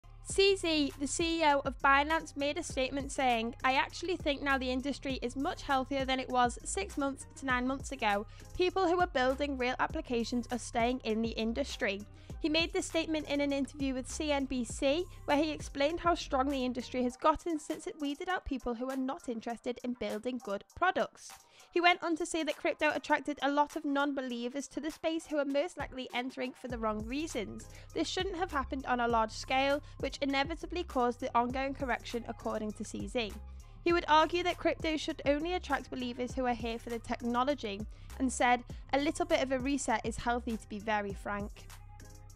"A RESET IS HEALTHY" Changpeng Zhao
NEW: Crypto News Update! Welcome back to the most efficient Cryptocurrency News Show.

Crypto has been cleansed says Binance CEO 🌱

Changpeng Zhao (CZ) went on to say that crypto attracted a lot of non-believers to the space who are most likely entering for the wrong reasons. This shouldn’t have happened on a large scale, which inevitably caused the ongoing correction according to CZ.

CZ would argue that crypto should only attract believers who are here for the technology. “A little bit of a reset is healthy to be very frank,”